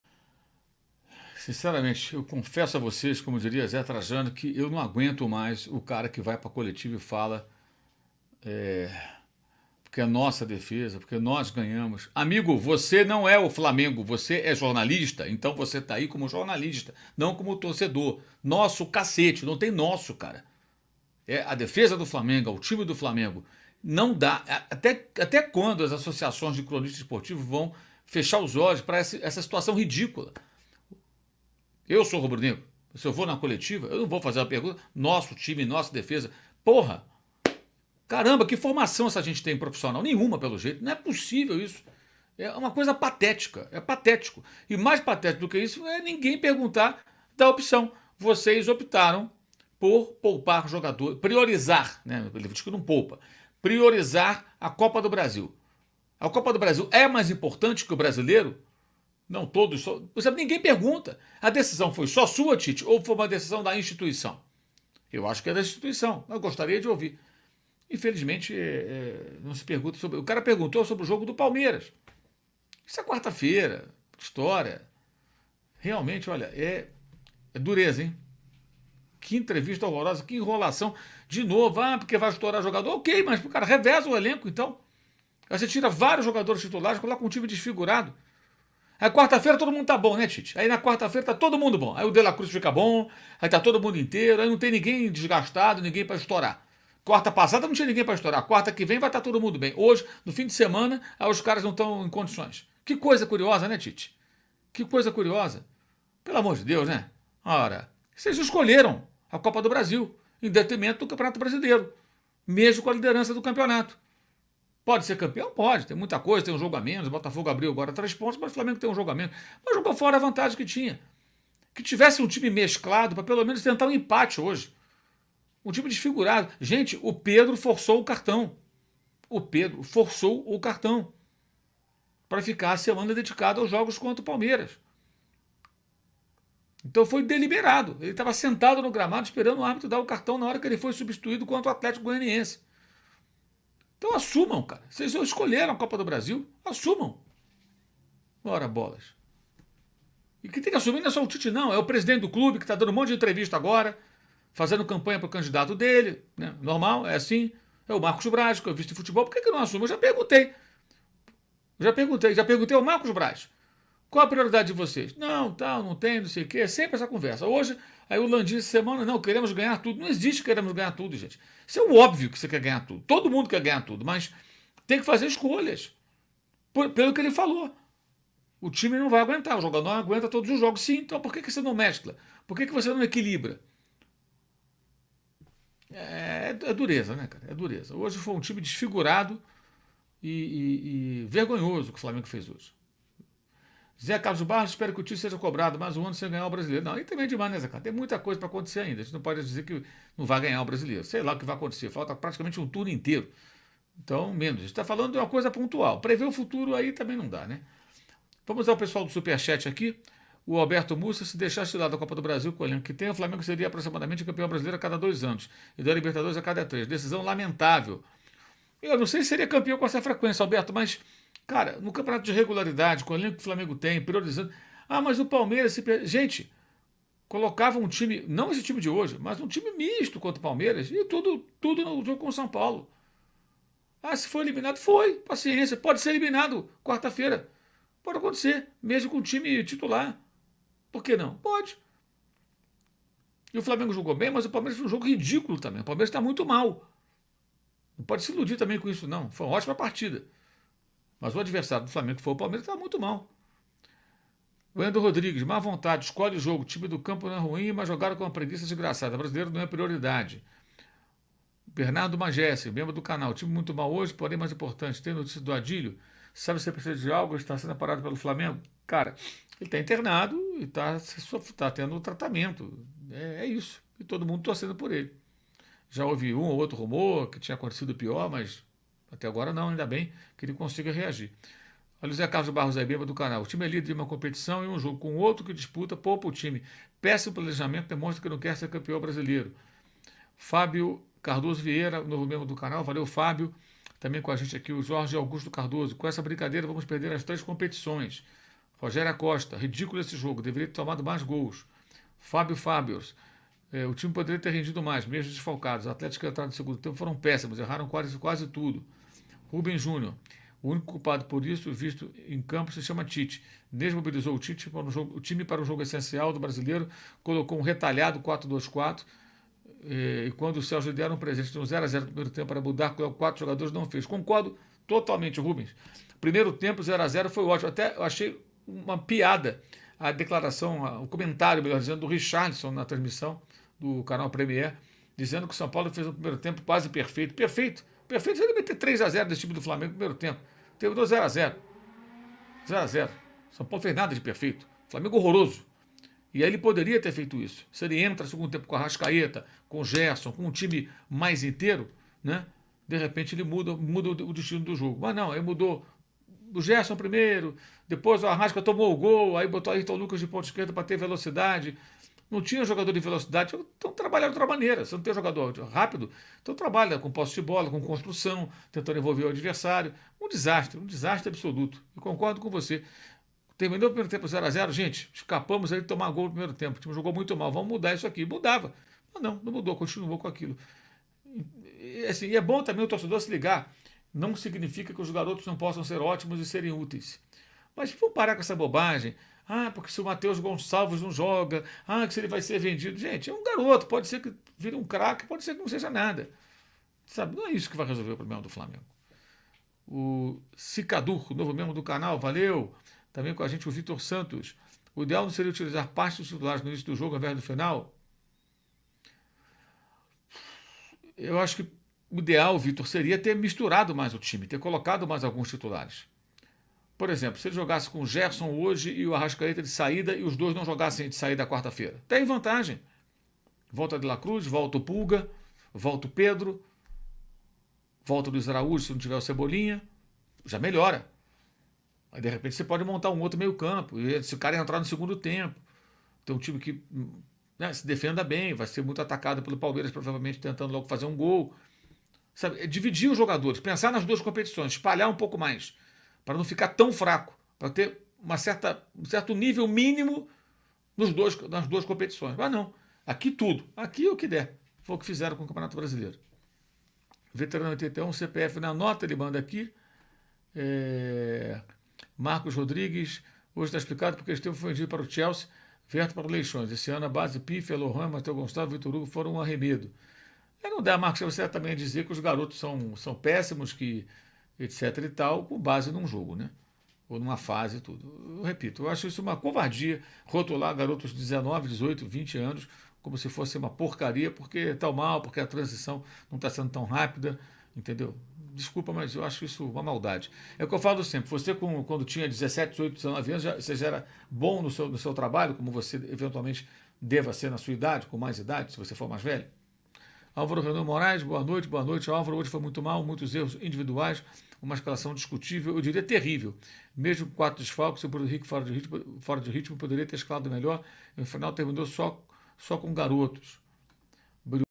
Flamengo prioriza a Copa do Brasil e despreza o campeonato Brasileiro: absurdo
Live no canal YouTube.com/MauroCezar​,03 de Agosto de 2024, depois de São Paulo 1 x 0 Flamengo